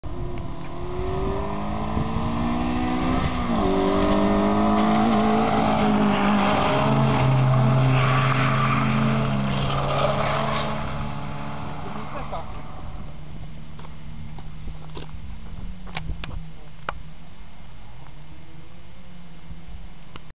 Suzuki Swift goni Fiata CC:)
Suzuki Swift goni Fiata CC podczas IV Rundy SMT na Torze Fiata 2007